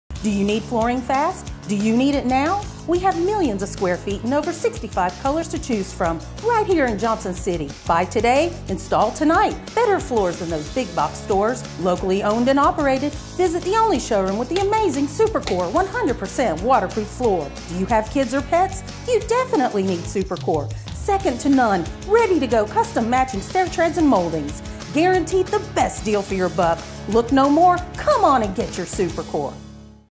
WeShipFloors SUPERFAST local commercial
Room Visualizer put Supercore in your Space!
Free Design Consultation - We help you get your project started and provide amazing how-to tips and tricks!

SUPERCore Xtreme is a revolution in waterproof flooring construction brought to you exclusively by WeShipFloors.com.  The KEY to SUPERCore being so super tough is it’s SPC (Stone Plastic Composite) core - guaranteed dimensionally stable from -30° F up to 155° F.  Scratch resistance, most SUPERCore waterproof flooring products have a 30 mil COMMERCIALLY rated wear layer. That’s thicker than most of the competition. We would add more wear layer – but it’s hard to add to a “lifetime warranty” finish.